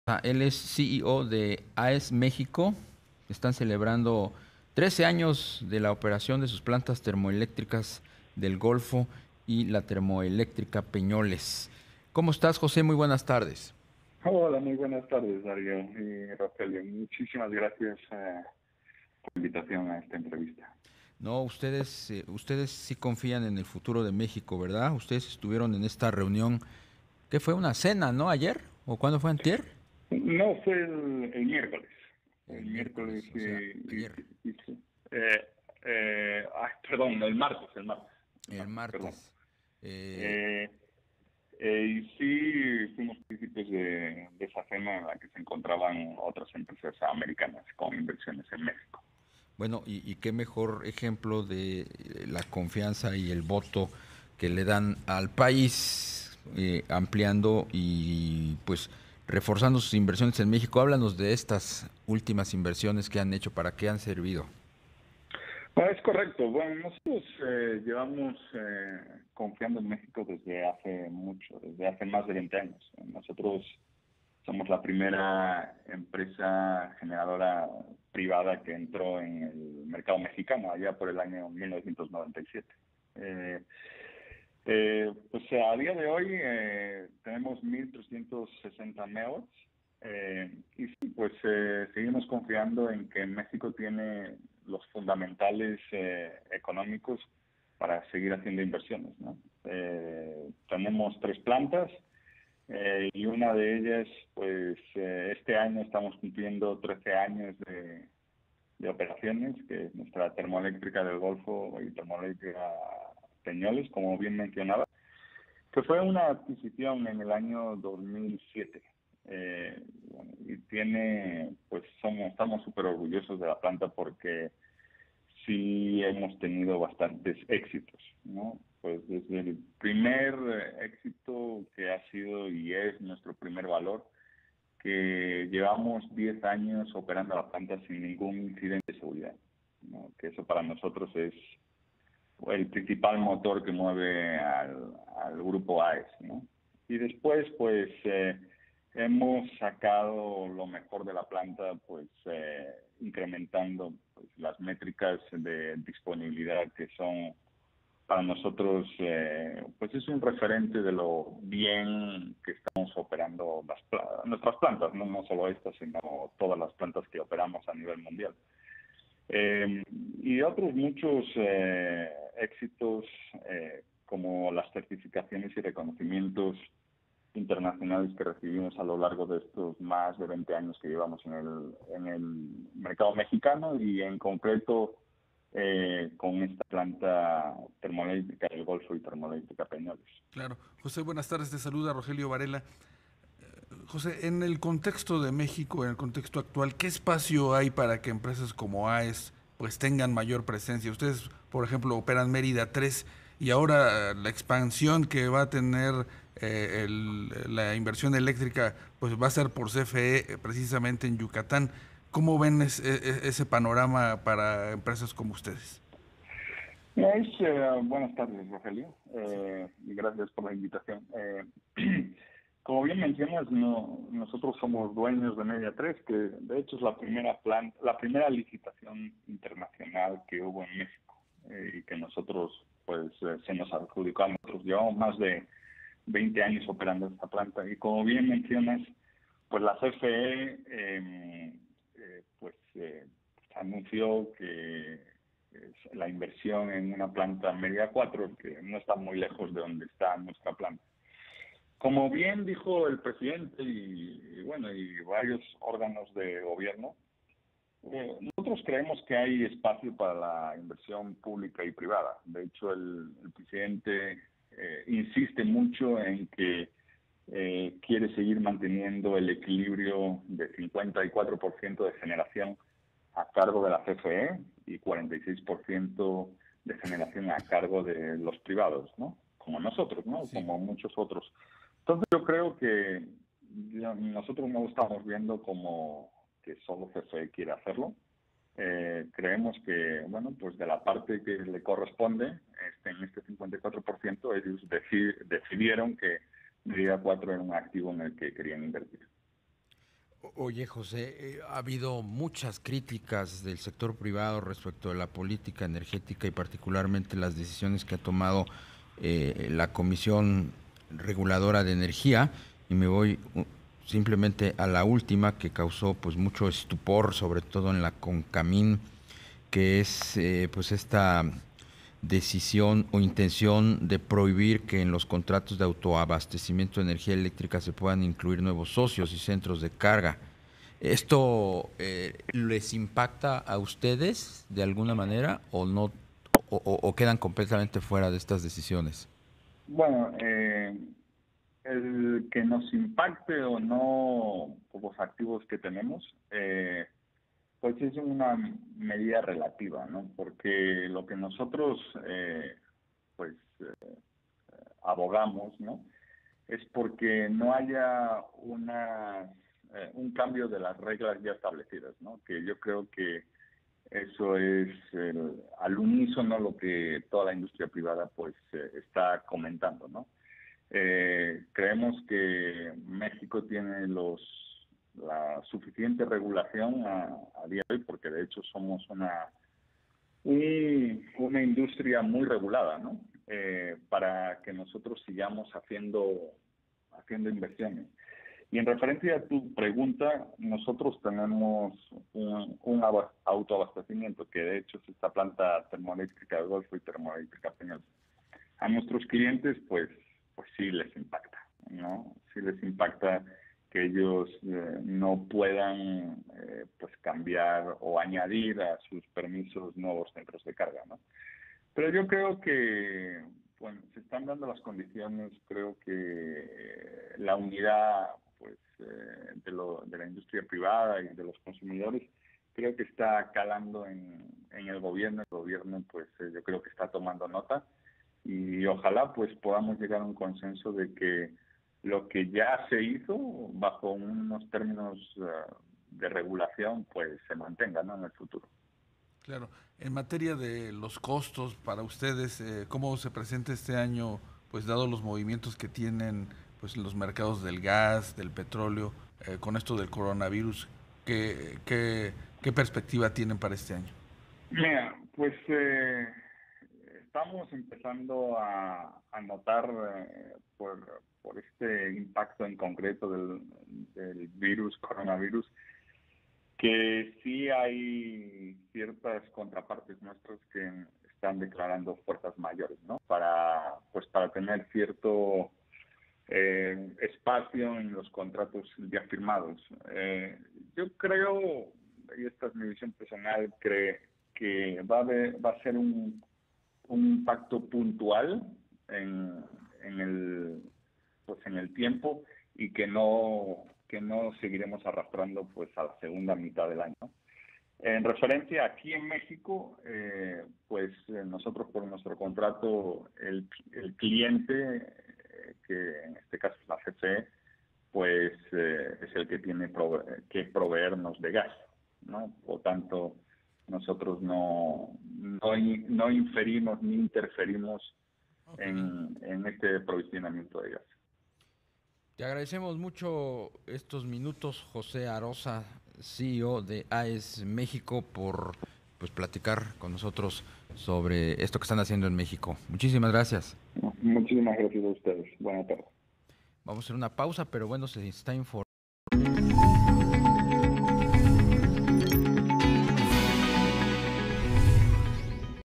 José Arosa habla de las inversiones de AES en México
José Arosa, CEO de AES México, habla en entrevista sobre las inversiones que realizarán y la celebración de los 13 años de operación en el país.

20 febrero de 2020